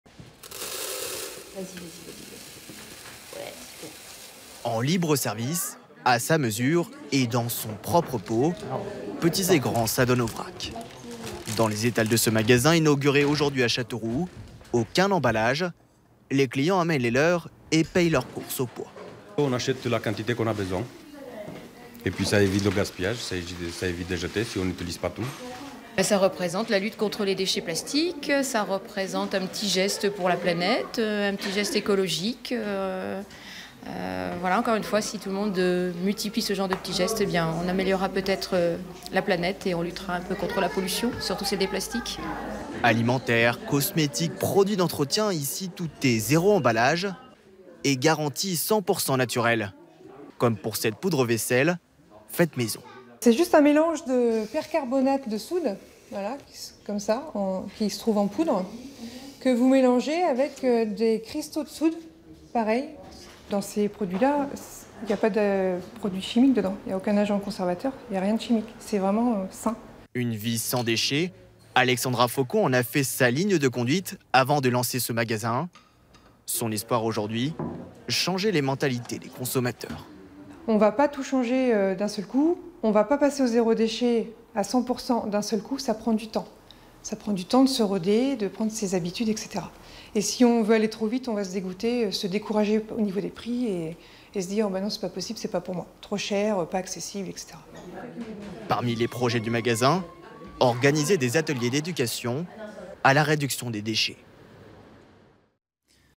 Châteauroux : ouverture d'une épicerie en vrac
Elles étaient 18 fin 2015, elles sont plus de 200 aujourd'hui. Les épiceries vrac connaissent un vrai succès partout en France. Et notre région ne fait pas exception. A Châteauroux, la première a ouvert ses portes ce matin. Plus qu'une épicerie, c'est aussi un atelier à produits ménagers.